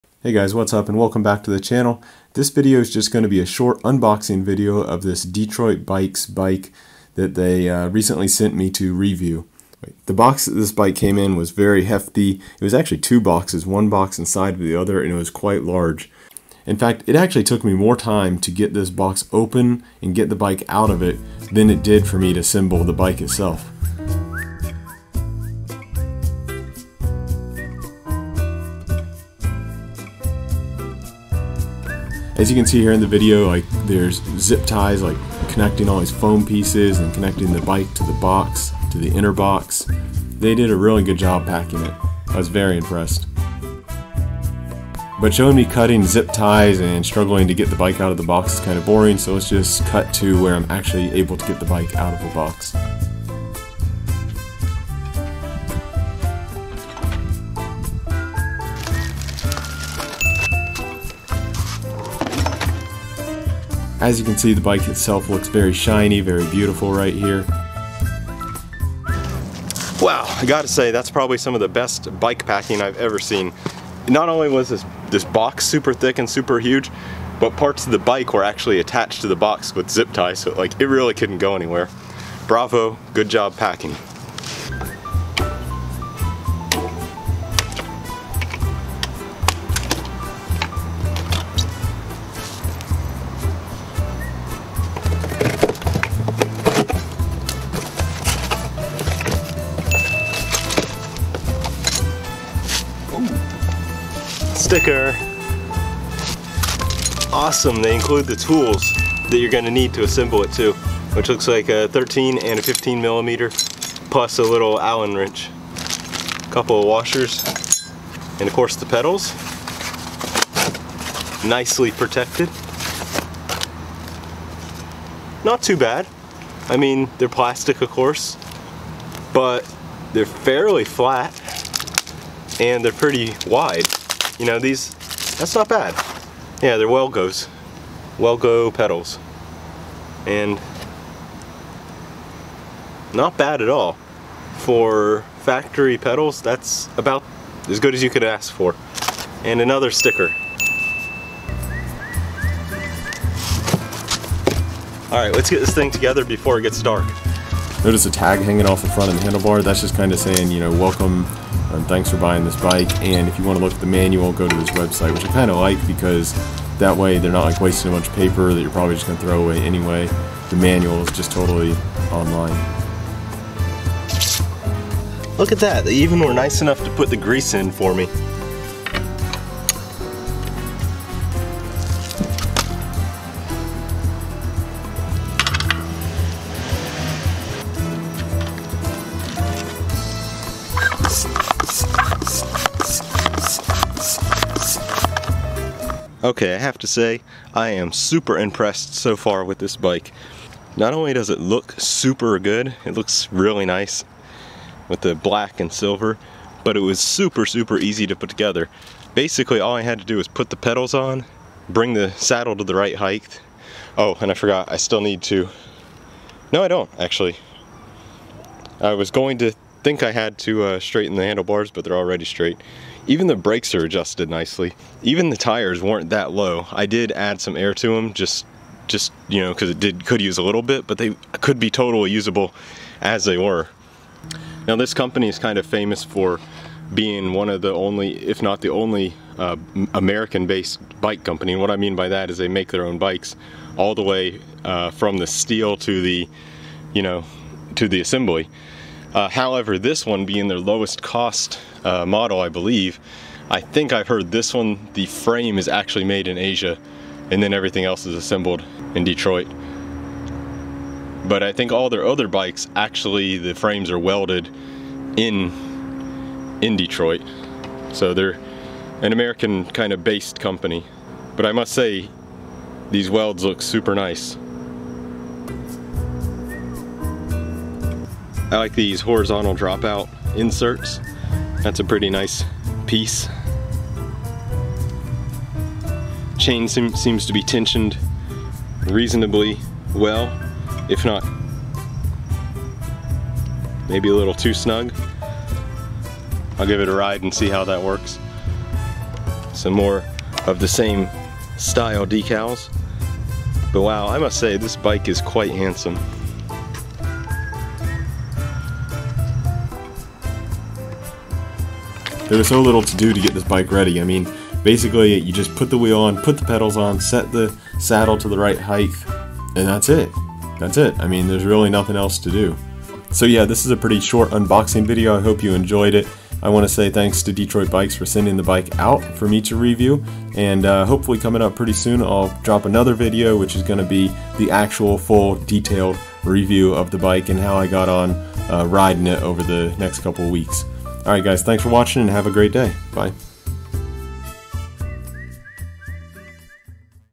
Detroit Sparrow - unboxing and assembly
In this short video I unbox and assemble Sparrow that Detroit Bikes sent me to review. We'll take a look at the packaging, the assembly process and overall looks. I'll save the details for my next video which will be the full review.